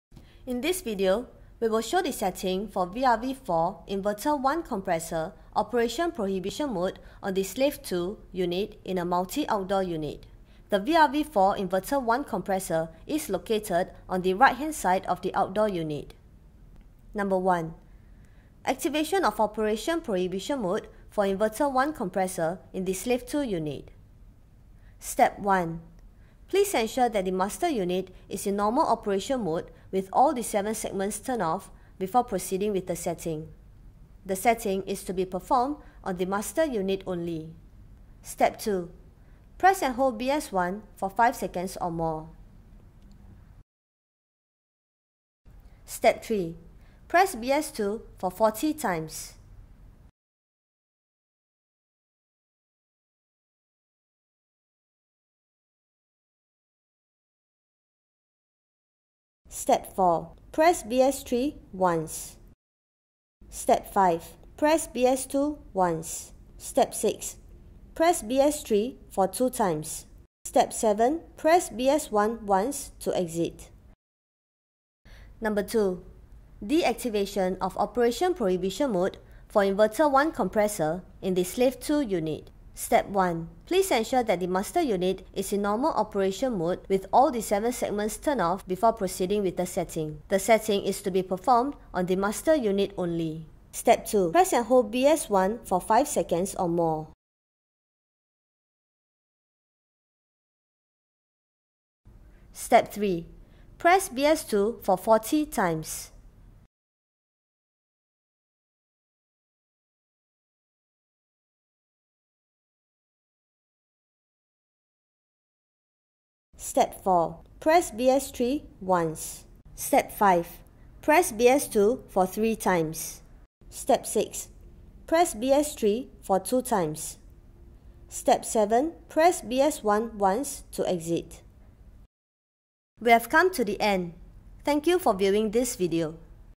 Emergency Setting for VRV IV Slave 2 Unit Inverter 1 Compressor Failure
Field settings can be conducted on the CU main PCB
To isolate the faulty inverter 01 compressor in the Slave2 Outdoor Unit while allowing other compressors to operate.
Applicable for VRV IV